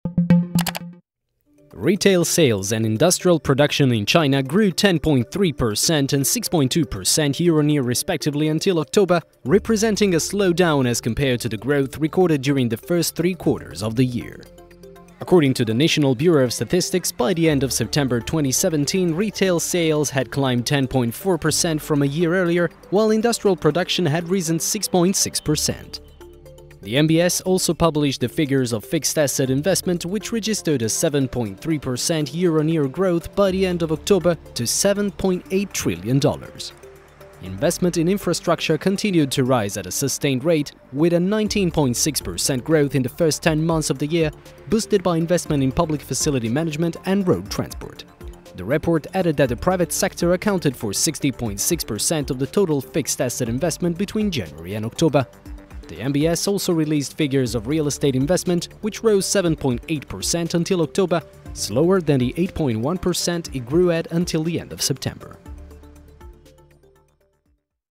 Industrial production, retail sales slow in China
Beijing (China), Nov 14 (EFE/EPA). Retail sales and industrial production in China grew 10.3 percent and 6.2 percent year-on-year respectively until October, representing a slowdown as compared to the growth recorded during the first three quarters of the year, the National Bureau of Statistics said Tuesday.

INCLUDES ARCHIVE FOOTAGE COURTESY OF HELLOFOOTAGE.COM.